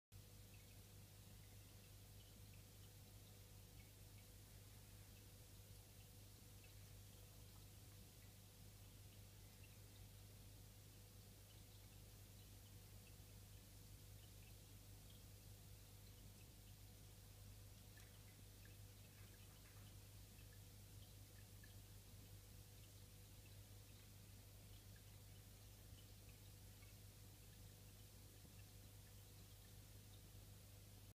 Fish Tank Experiment
My daughter’s fish Spottless who used to be called Spot until he lost his spots had his tank clean and I’ve added some sticks for a different environment.
Waiting to see how they take shape as algae grows and starts looking more natural.
So far he’s liking the new look and having fun swimming around.